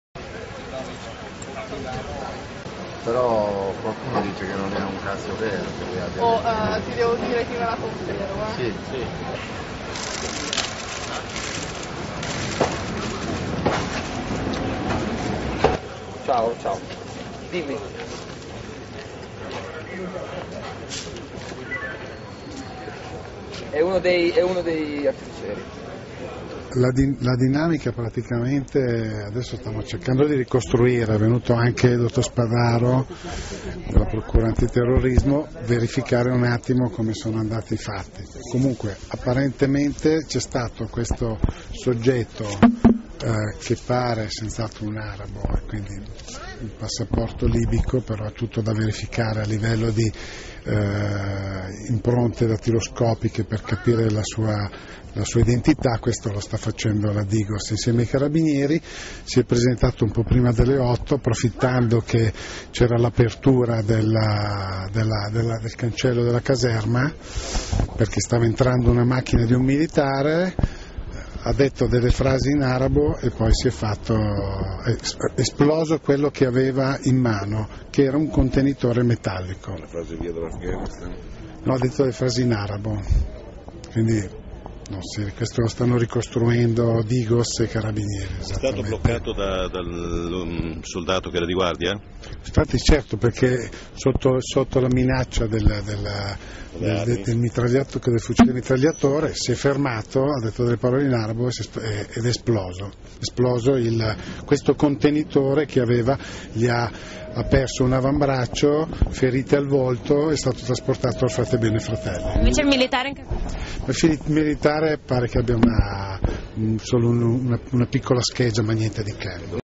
Attentato alla Caserma Santa Barbara di Milano
Milano. Alle ore 7.45, un uomo, forse di origini libiche, ha fatto esplodere un ordigno davanti alla Caserma Santa Barbara di Piazzale Perucchetti. Il soldato che gli ha intimato l'alt, Guido La Veneziana, è rimasto lievemente ferito mentre l'attentatore ha perso l'avambraccio ed è ora ricoverato al Fatebenefratelli. Tullio Mastrangelo, Comandante Polizia Locale di Milano. Servizio di Claudia Bellante